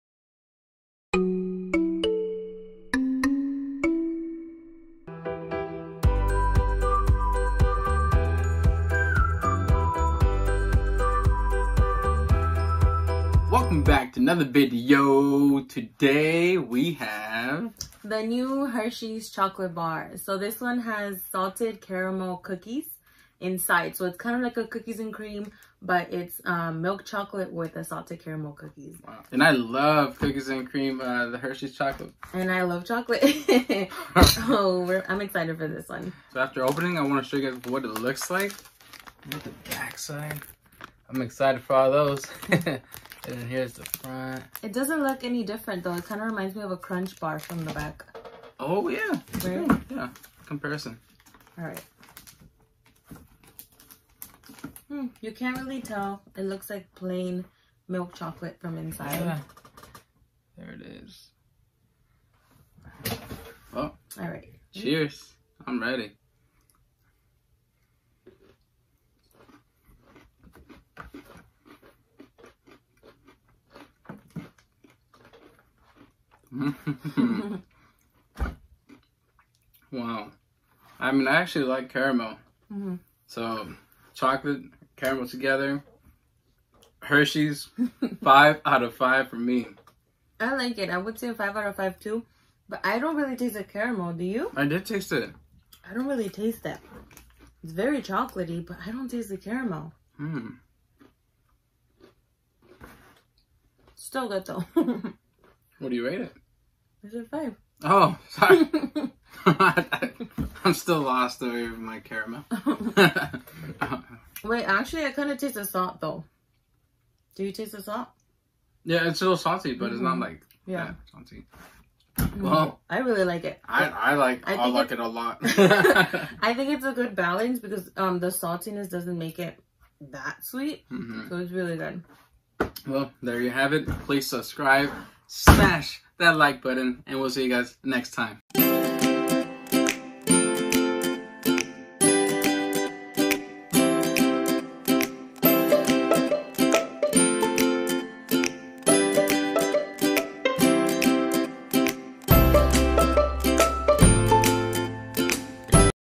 New Hershey's Milk Chocolate with Salted Caramel Cookies Review!!! 🍫🧂🍪
NEW Hershey's Milk Chocolate with Salted Caramel Cookies!!!

Hershey's Chocolate finally came out with a delicious chocolate bar filled salted caramel cookies!